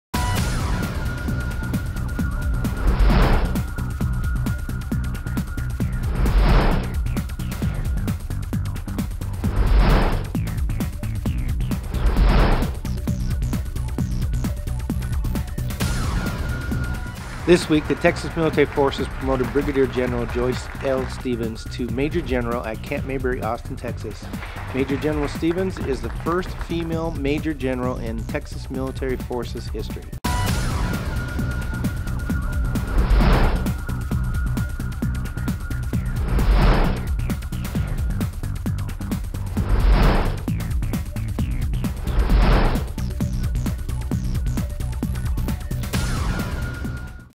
Major General Joyce L. Stevens
This week the Texas Military Forces promoted Brigadier General Joyce L Stevens to Major General at Camp Mabry Austin Texas. Major General Stevens is the first female Major General in Texas Military Forces history.

Produced by Texas Military Forces